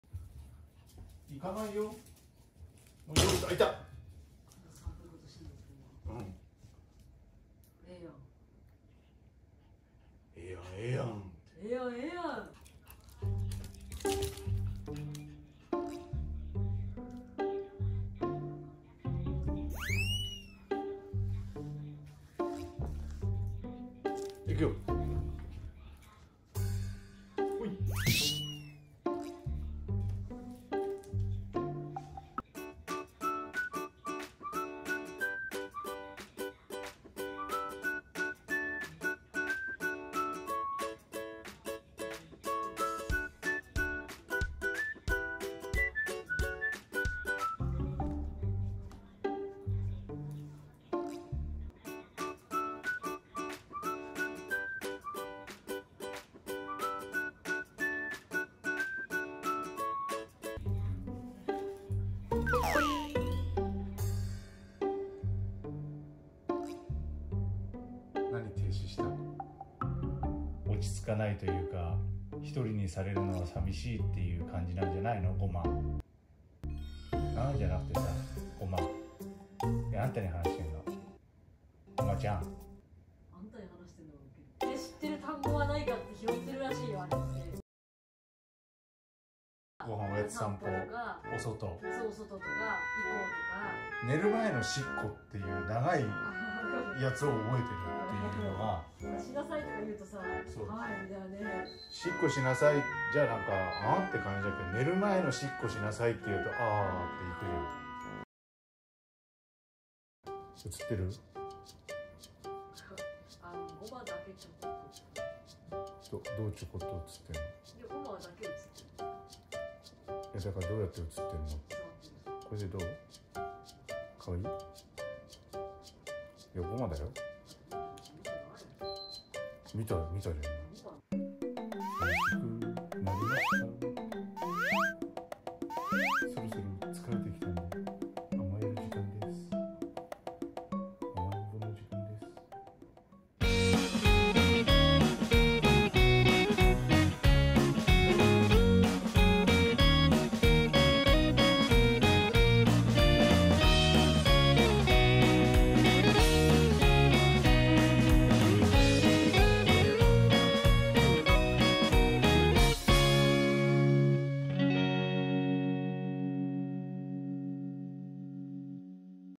犬って飼い主の話をしっかり聞いて行動も見てるよねっていうお話。
以上、犬って飼い主の話をしっかり聞いて行動も見てるよねっていうお話でした。